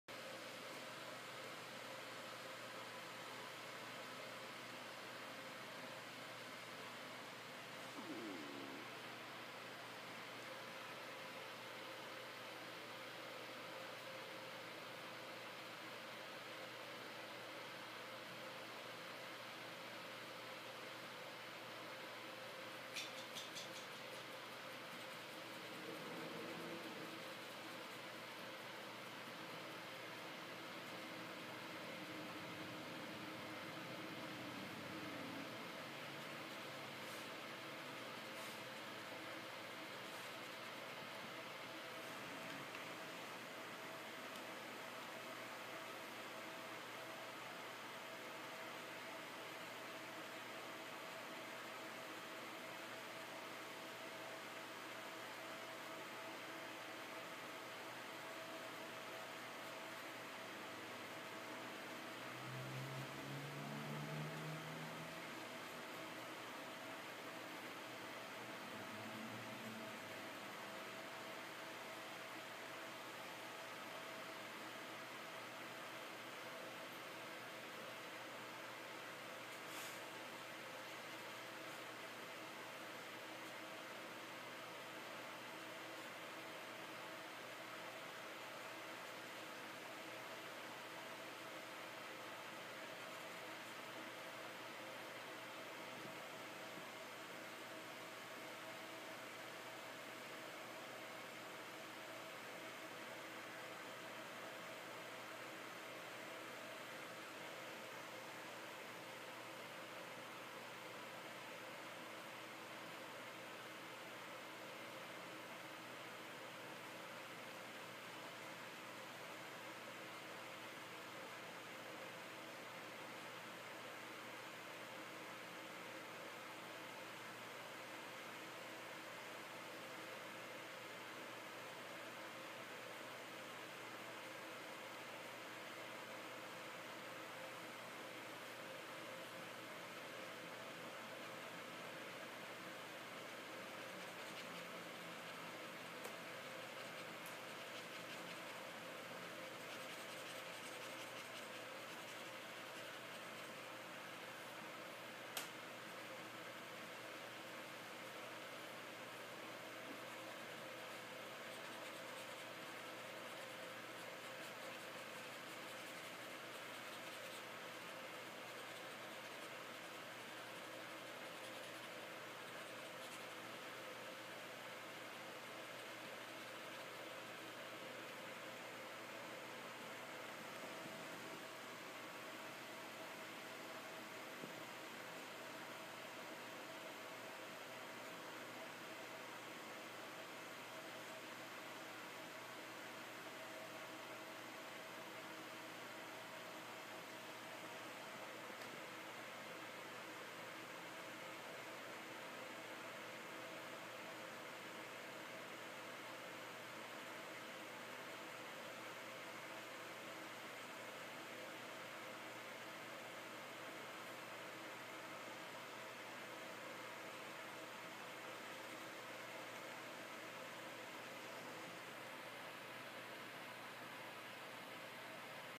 1 Digital painting (chalk and oil) of my cat and dog - Painter X3
This is a few minutes of watching me paint on my "digital easel" a digital painting if my cat Olive and dog Duncan cuddling (Part 1)